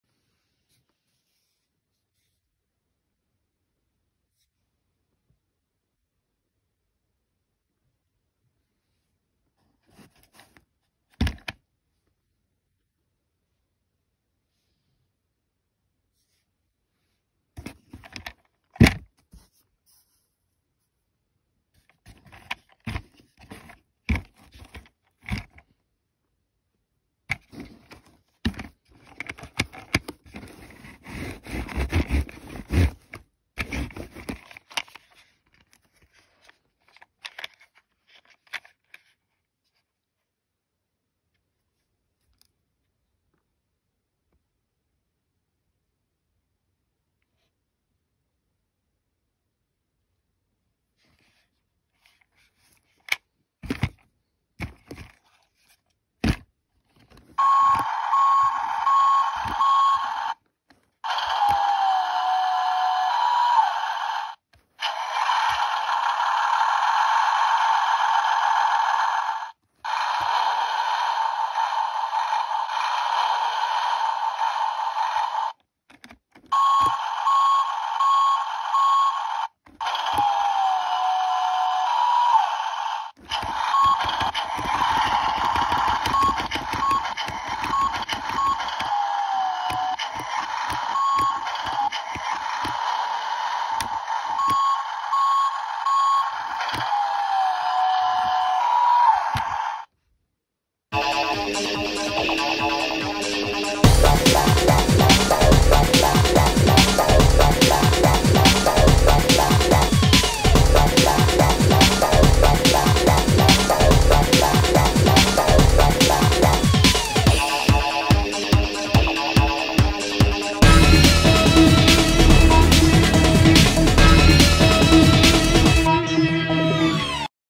2008 Tonka Funrise Toughest Minis Bulldozer
Where I got it: Somewhere when I was young

Condition: Almost perfect, possibly a few scratches